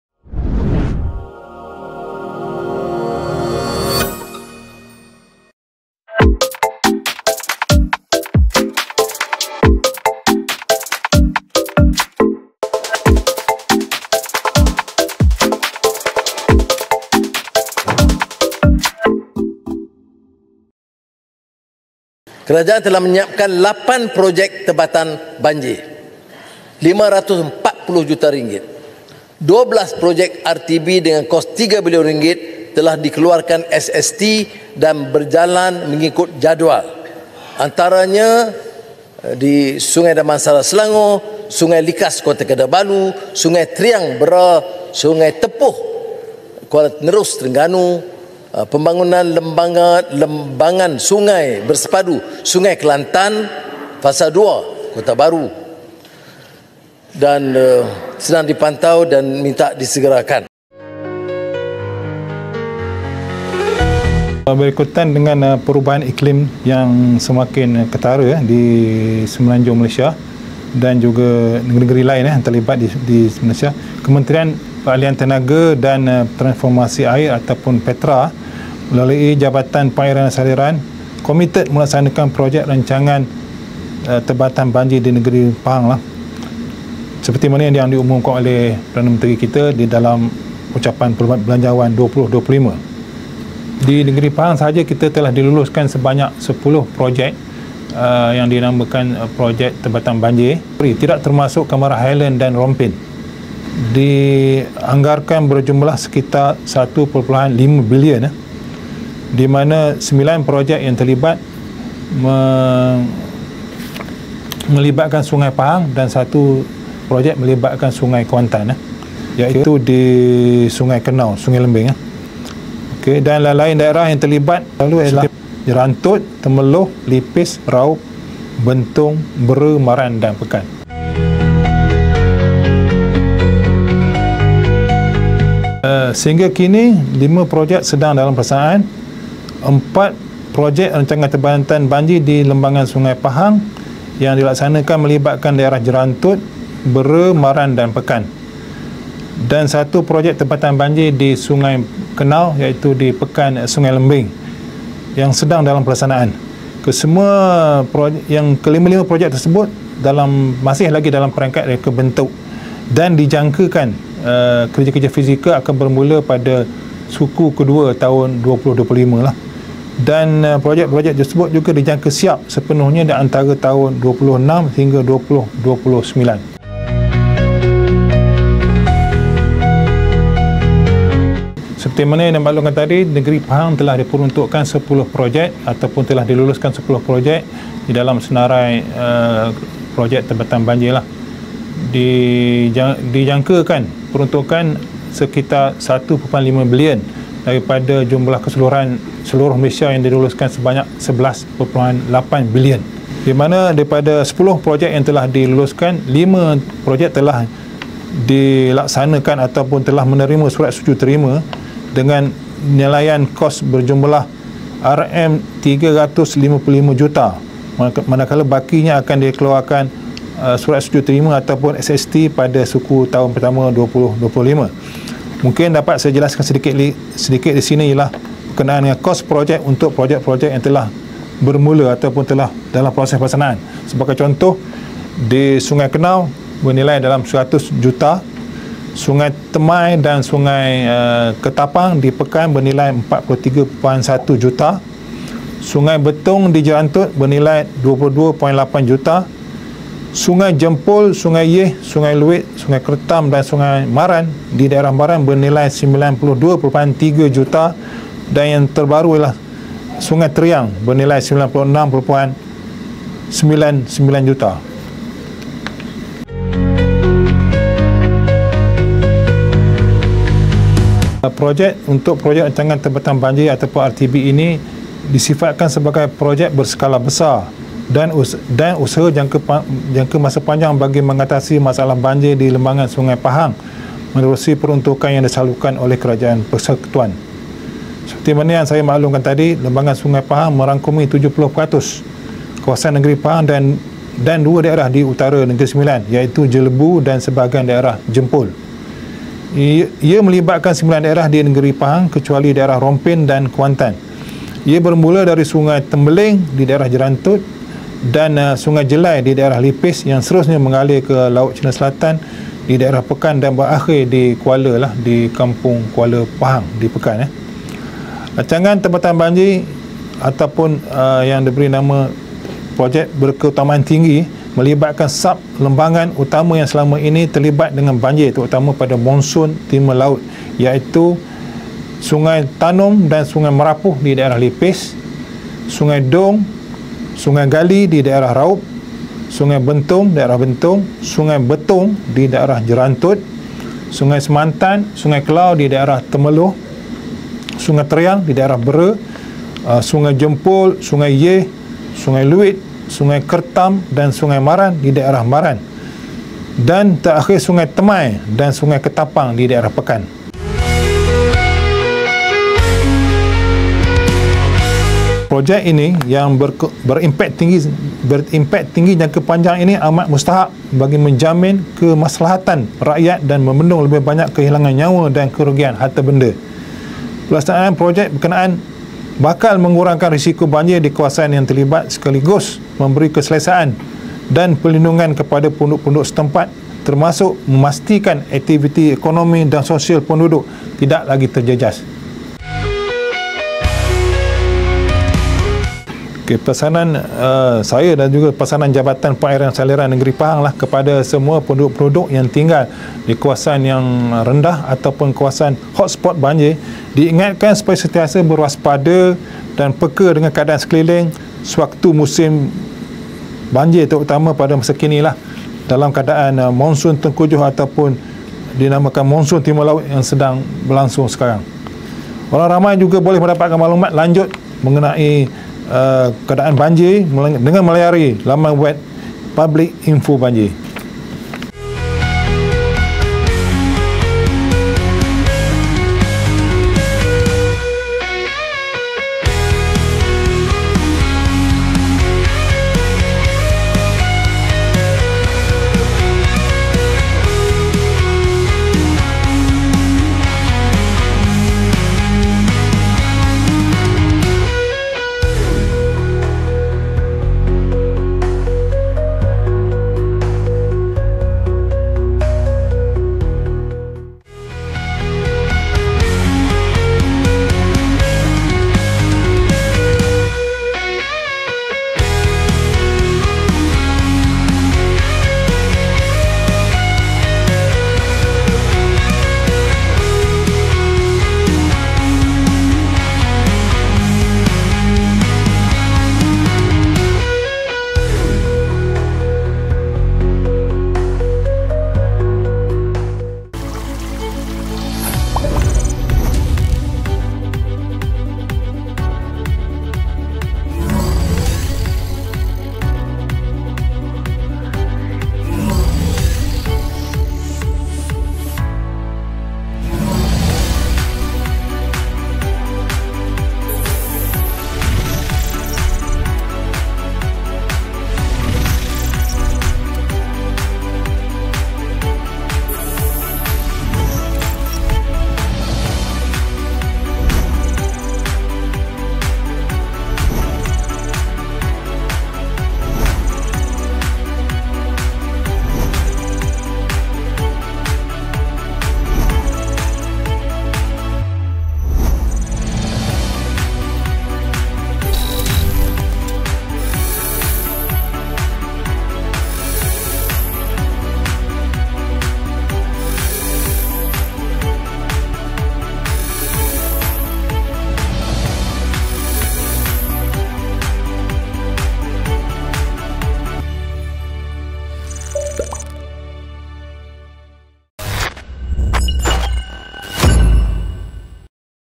Influencer@JaPen Edisi Khas Belanjawan 2025 | "Projek Tebatan Banjir Di Pahang"
Jom saksikan Influencer @Japen Edisi Khas "Projek Tebatan Banjir Di Pahang di saluran Youtube dan Facebook Jabatan Penerangan Negeri Pahang pada 27 November 2024 (Rabu), jam 11.00 pagi membicarakan berkenaan dengan 'Projek Tebatan Banjir Negeri Pahang'

Panel Jemputan:
YBhg Tuan Ir. Mohd. Sha'ri Bin Mohammed,
Pengarah Jabatan Pengairan & Saliran Negeri Pahang.

@Jabatan Penerangan Pahang

Editor : Izzat Abd Rahman
Video : Fazli Zulkifli/Azhan Mahayuddin/RTM/JPS Malaysia